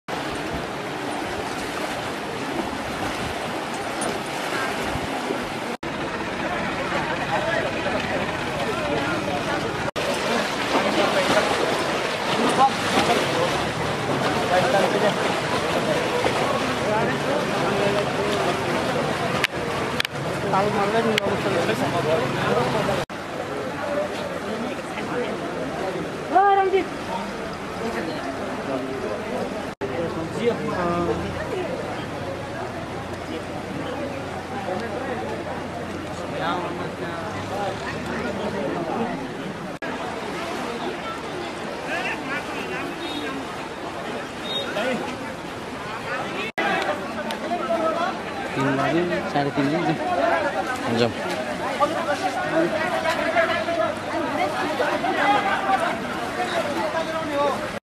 (You won't believe this) Elephant rescue hundreds of tourists from a flooded Safari Park in Nepal
Elephants rescue hundreds of tourists from a flooded safari park in Nepal amid flash floods and landslides.

For latest Comedy, News and lots more.